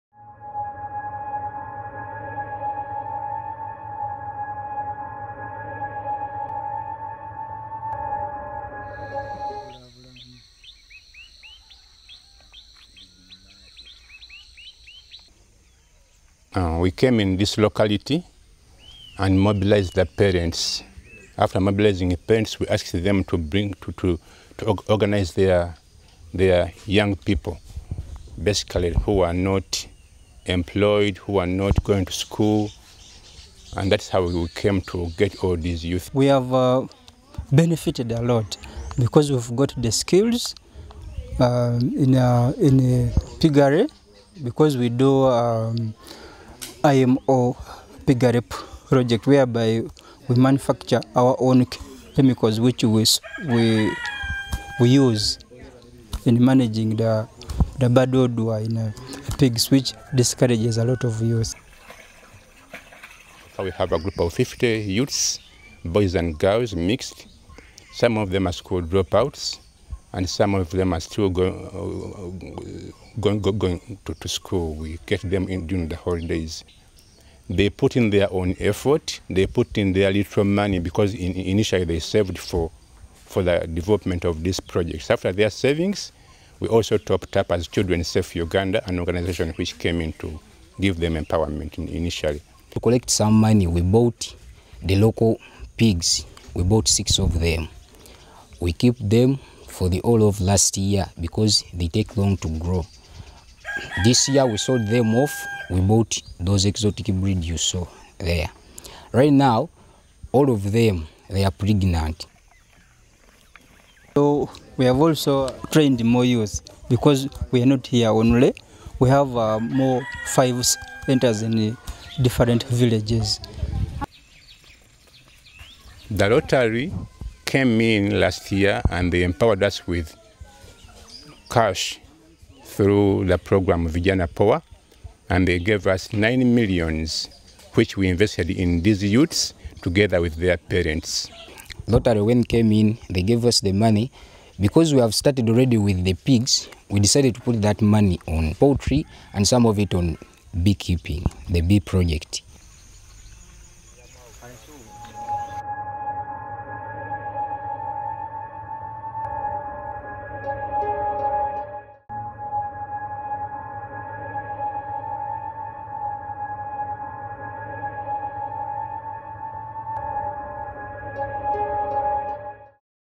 RC Kasangati Rotary Vijana Poa projects
This video shares highlights from the Rotary Club of Kasangati, which has supported 50 youth to find employment under the Rotary Vijana Poa; Youth into Work Initiative.